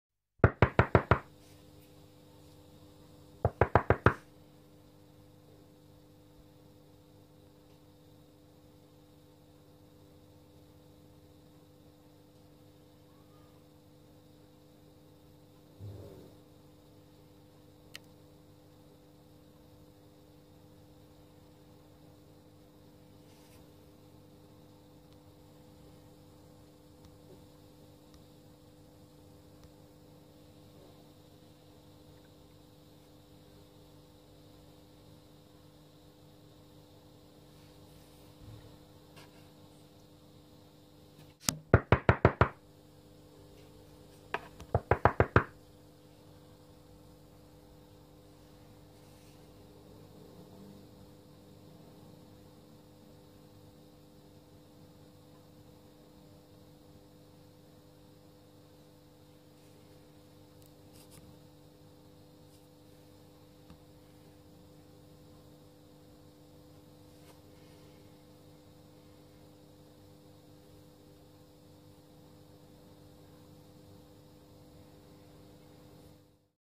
Curious Red Fish
The Red Fish fish goes to see who is knocking on the glass